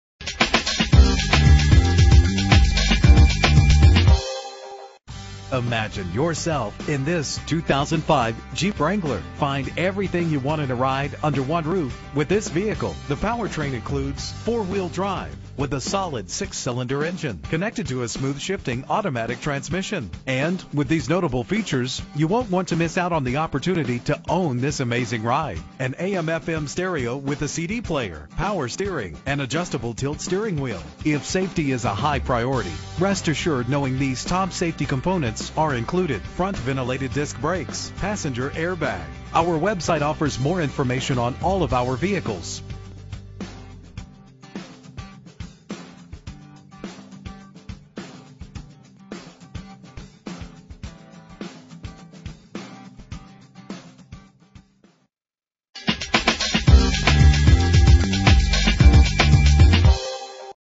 2005 Jeep Wrangler - Jackson MI
Find Similar Vehicles:
Year: 2005
Make: Jeep
Model: Wrangler
Trim: X
Engine: 4.0 liter inline 6 cylinder 12 valve
Transmission: 4-Speed Automatic
Color: Blue
Mileage: 36397
Address:
2200 Seymour Road